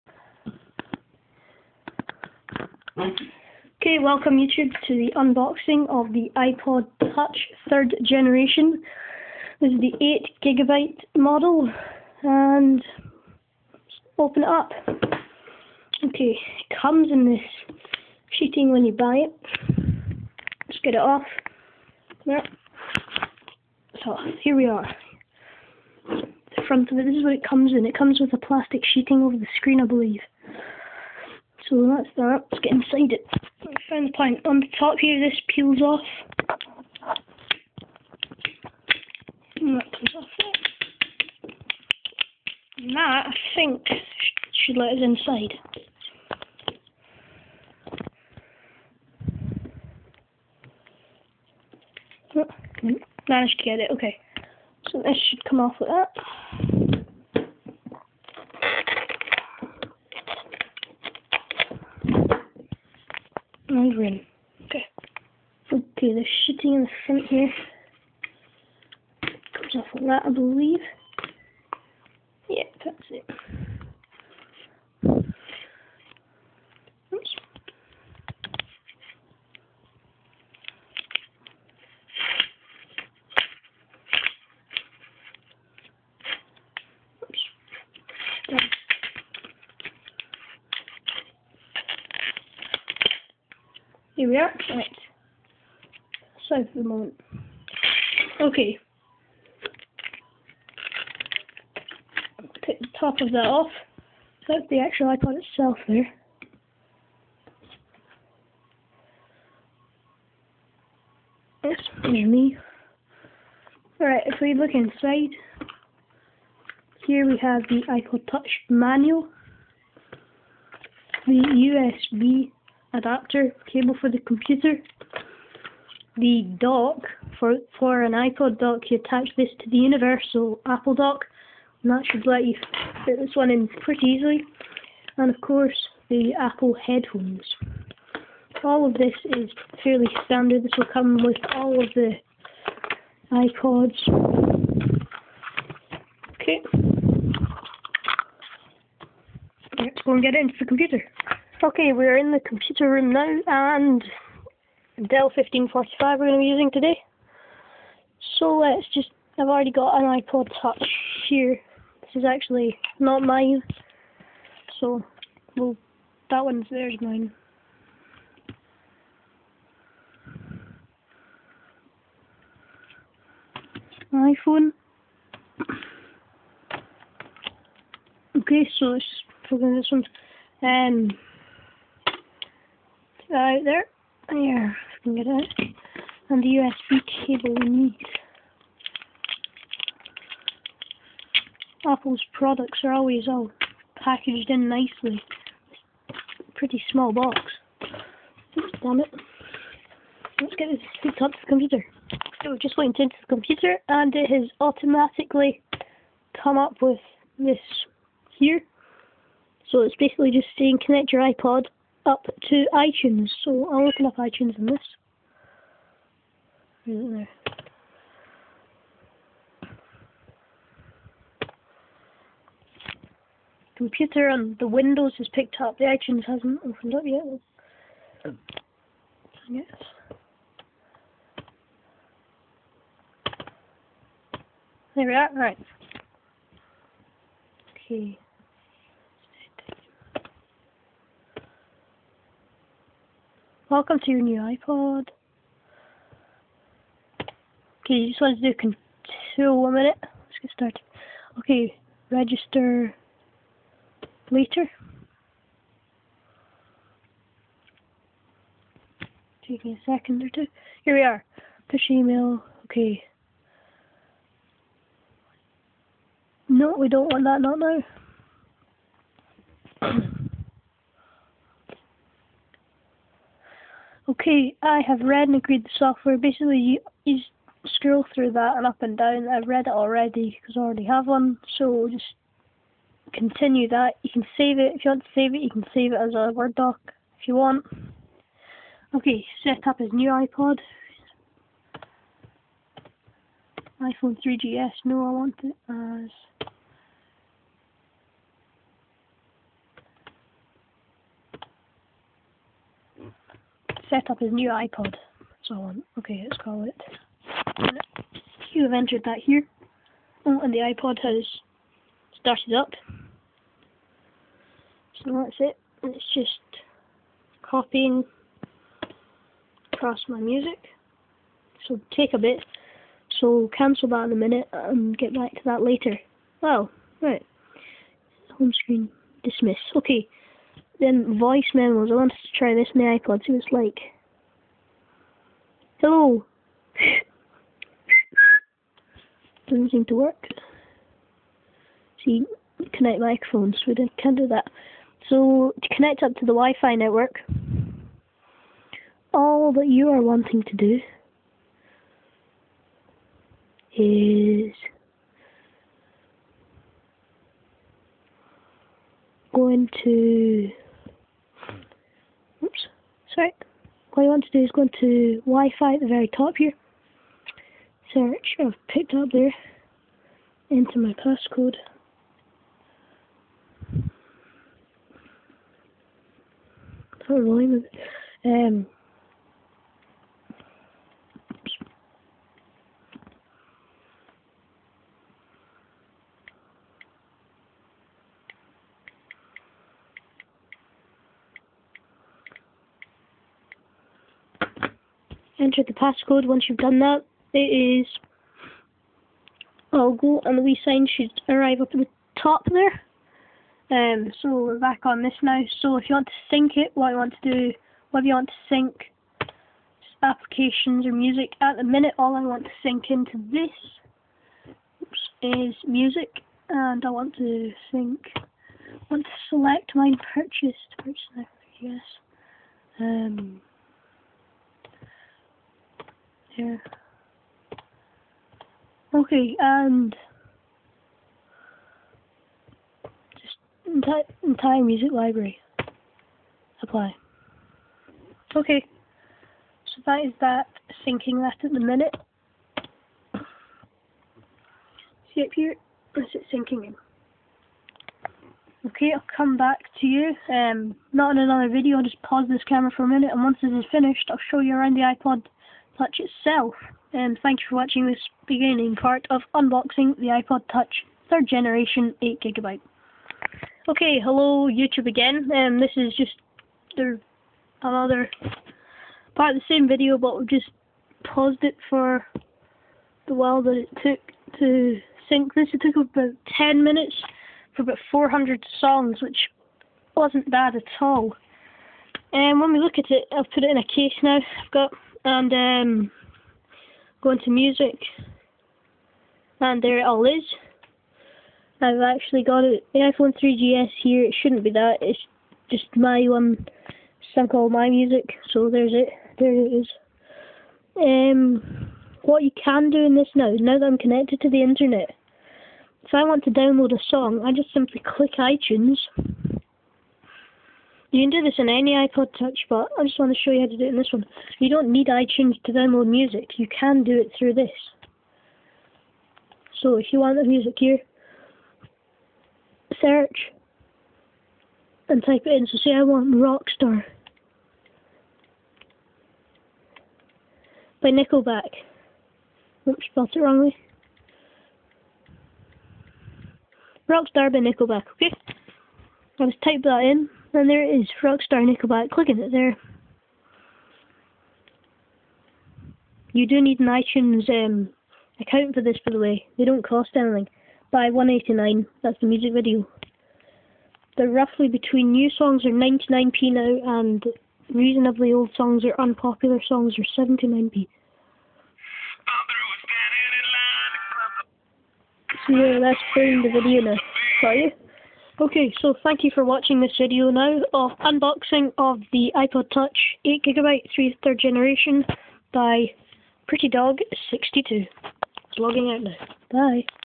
Unboxing of ipod touch 3rd generation
Another unboxing from prettydog62
Of the 8gig ipod touch 3rd generation (with bluetooth and speaker)